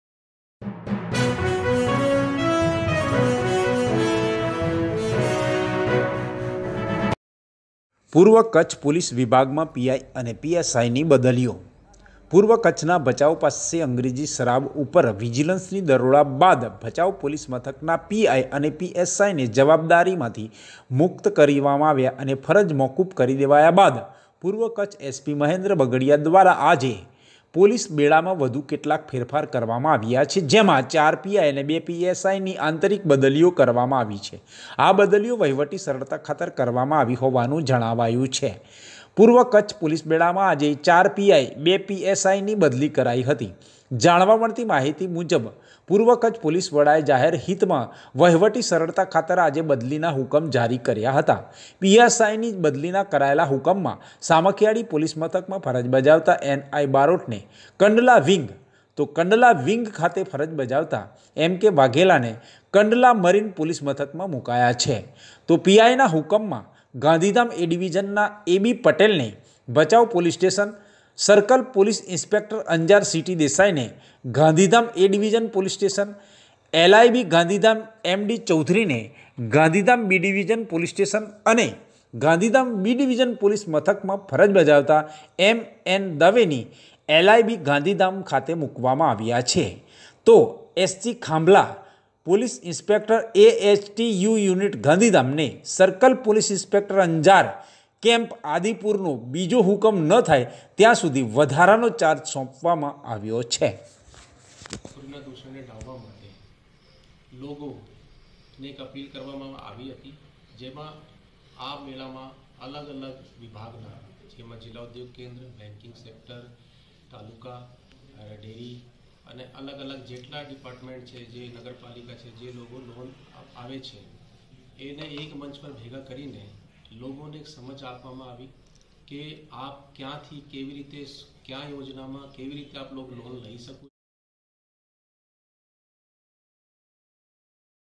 પૂર્વ કચ્છ પોલીસ વિભાગમાં પીઆઈ અને પીએસઆઇ ની બદલીઓ 4 પીઆઈ અને 2 પીએસઆઈની આંતરિક બદલી
𝗟𝗮𝘁𝗲𝘀𝘁 𝗡𝗲𝘄𝘀
પૂર્વ કચ્છ પોલીસ વિભાગમાં પીઆઈ અને પીએસઆઇ ની બદલીઓ

પૂર્વ કચ્છના ભચાઉ પાસે અંગેજી શરાબ ઉપર વિજીલન્સની ટીમના દરોડા બાદ ભચાઉ પોલીસ મથકના પીઆઇ અને પીએસઆઇ ને જવાબદાર માની ફરજ મોકૂફ કરી દેવાયા બાદ પૂર્વ કચ્છ એસપી મહેન્દ્ર બગડીયા દ્વારા આજે પોલીસ બેડામાં વધુ કેટલાક ફેરફાર કરવામાં આવ્યા છે. જેમાં 4 પીઆઈ અને 2 પીએસઆઈની આંતરિક બદલી કરવામાં આવી છે. આ બદલીઓ વહીવટી સરળતા ખાતર કરવામાં આવી હોવાનું જાહેર કરાયું હતું.

પૂર્વ કચ્છ પોલીસ બેડામાં આજે 4 પી. આઈ., 2 પી. એસ. આઈ. ની બદલી કરાઈ હતી. જાણવા મળતી વિગતો મુજબ પૂર્વ કચ્છ પોલીસવડાએ જાહેરહિતમાં વહીવટી સરળતા ખાતર આજે બદલીના હુકમો જારી કર્યા હતા. પીએસઆઈની બદલીના કરાયેલા હુકમમાં સામખીયાળી પોલીસ મથકે ફરજ બજાવતા એન. આઈ. બારોટને કંડલા વિંગ, તો કંડલા વિંગ ખાતે ફરજ બજાવતા એમ. કે. વાઘેલાને કંડલા મરીન પોલીસ મથકમાં મુકાયા છે. તો પીઆઈનાં હુકમમાં ગાંધીધામ એ-ડીવીઝનના એ. બી. પટેલને ભચાઉ પોલીસ સ્ટેશન, સર્કલ પો. ઈન્સ. અંજાર સી. ટી. દેસાઈને ગાંધીધામ એ-ડીવીઝન પોલીસ સ્ટેશન, એલઆઈબી ગાંધીધામ એમ. ડી. ચૌધરીને ગાંધીધામ બી. ડીવીઝન પોલીસ સ્ટેશન અને ગાંધીધામ બી-ડીવીઝન પોલીસ મથકમાં ફરજ બજાવતા એમ. એન. દવેની એલઆઈબી ગાંધીધામ ખાતે મુકાયા છે. તો એસ. જી. ખાંભલા(પો. ઈન્સ. એ. એચ. ટી. યુ. યુનિટ ગાંધીધામ)ને સર્કલ પો. ઈન્સ. અંજાર(કેમ્પ આદિપુર)નો બીજો હુકમ ન થાય ત્યાં સુધી વધારાનો ચાર્જ સોંપવામાં આવ્યો છે.